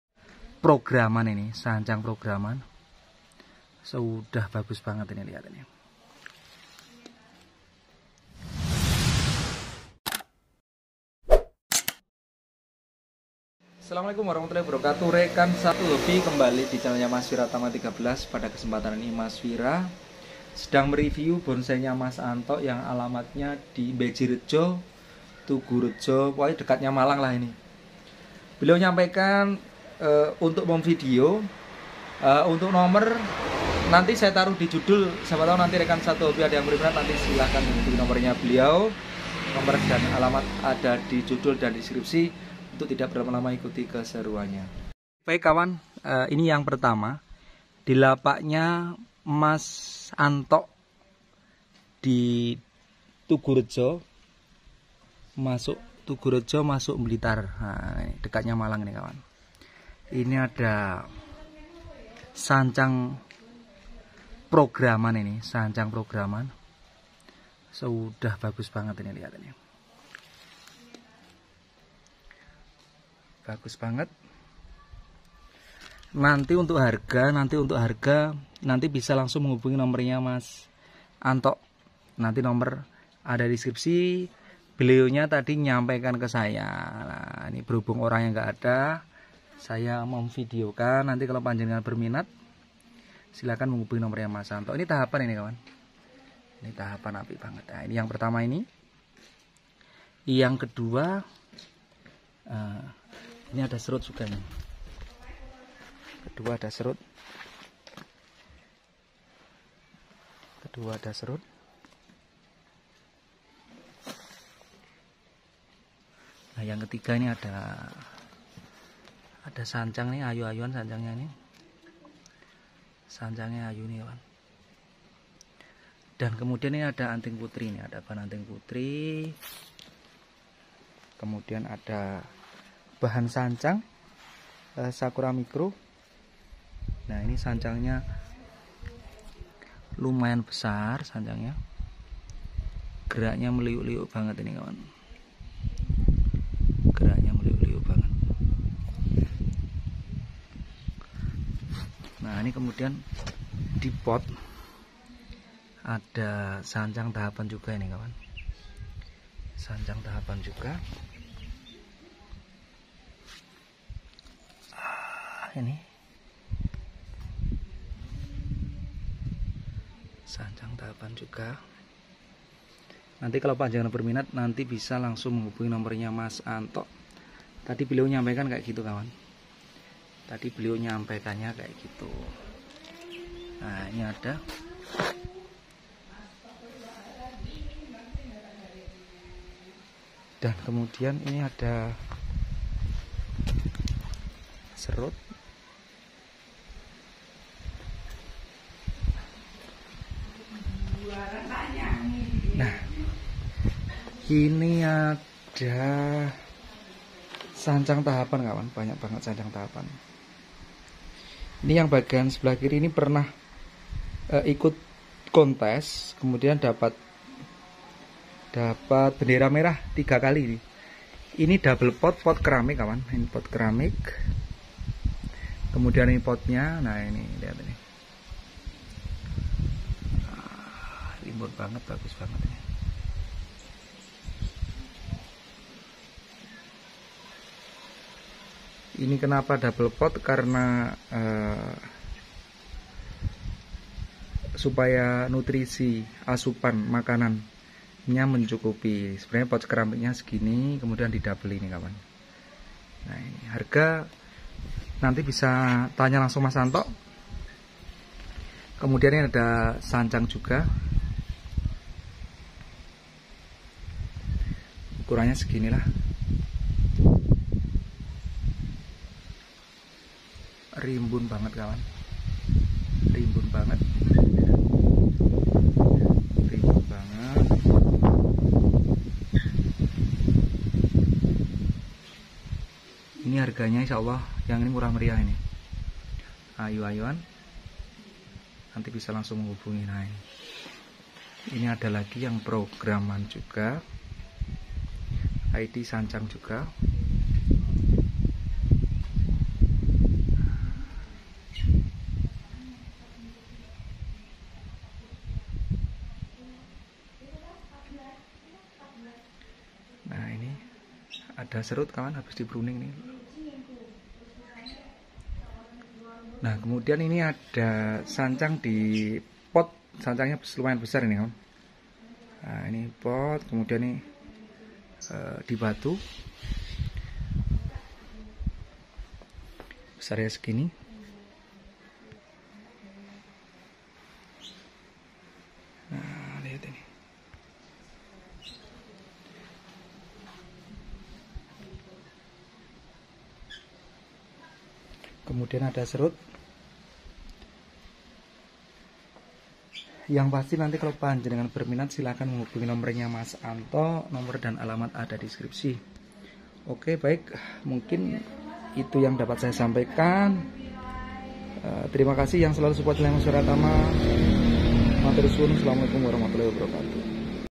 BONSAI TAHAPAN‼️CEK HARGA BONSAI DIRUMAH MAS ANTO ☎️+62821-3931-4775
Assalamualaikum warrahmatullhi wabbarakatu
selamat Siang
salam satu hobi
BONSAI TAHAPAN‼️CEK HARGA BONSAI DIRUMAH MAS ANTO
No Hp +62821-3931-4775
dengan Alamat Tugu Rejo Wates Blitar
Siapa tau rekan sehobi ada yang minat
Banyak yang baru pastinya

Cek harga bonsai di rumah Mbah Yo

Program akar beringin iprik